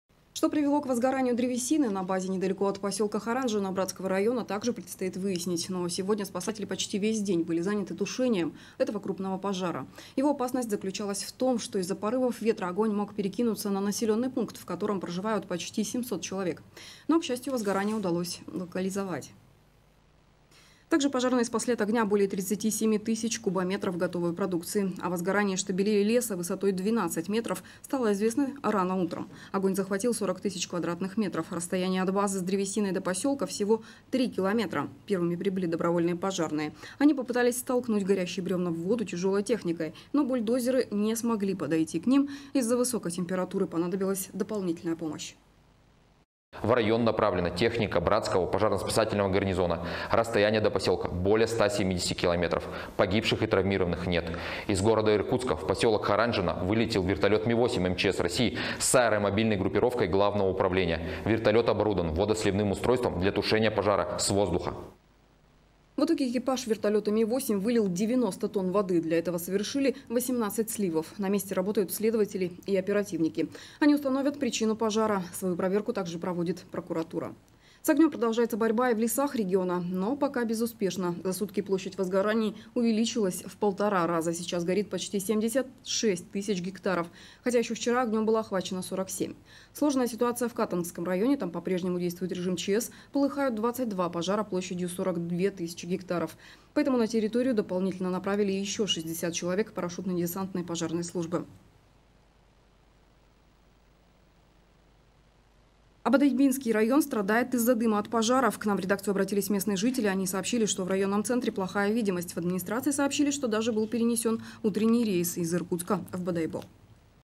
Пожар на лесозаготовительной базе едва не перекинулся на посёлок в Братском районе
Что привело к возгоранию древесины на базе недалеко от посёлка Харанжино Братского района, также предстоит выяснить. Но сегодня спасатели почти весь день были заняты тушением этого крупного пожара. Его опасность была в том, что из-за порывов ветра огонь мог перекинуться на населённый пункт, в котором проживают почти 700 человек. Но, к счастью, возгорание удалось локализовать.

Также пожарные спасли от огня более 37 тысяч кубометров готовой продукции. О возгорании штабелей леса высотой 12 метров стало известно рано утром. Огонь захватил сорок тысяч квадратных метров. Расстояние от базы с древесиной до посёлка всего три километра. Первыми прибыли добровольные пожарные. Они попытались столкнуть горящие брёвна в воду тяжёлой техникой. Но бульдозеры не смогли подойти к ним из-за высокой температуры. Понадобилась дополнительная помощь.

АНДРЕЙ ШУТОВ, начальник пресс-службы ГУ МЧС России по Иркутской области:

«В район направлена техника братского пожарно-спасательного гарнизона. Расстояние до посёлка более 170 километров. Погибших, травмированных нет. Из города Иркутска в посёлок Харанжино направлен вертолёт Ми-8 МЧС России с аэромобильной группировкой специализированной пожарно-спасательной части. Вертолёт оборудован водосливным устройством для тушения пожара с воздуха».

В итоге экипаж вертолёта Ми-8 вылил 90 тонн воды. Для этого совершили 18 сливов. На месте работают следователи и оперативники. Они установят причину пожара. Свою проверку также проводит прокуратура.

С огнём продолжается борьба и в лесах региона. Но пока безуспешно. За сутки площадь возгораний увеличилась в полтора раза. Сейчас горит почти 76 тысяч гектаров, хотя ещё вчера огнём было охвачено 47 тысяч. Сложная ситуация в Катангском районе. Там по-прежнему действует режим ЧС. Полыхают 22 пожара площадью 42 тысячи гектаров. Поэтому на территорию дополнительно направили ещё 60 человек парашютно-десантной пожарной службы.

А Бодайбинский район страдает от дыма от пожаров. К нам в редакцию обратились местные жители. Они сообщили, что в районном центре плохая видимость. В администрации сообщили, что даже был перенесён утренний рейс из Иркутска в Бодайбо.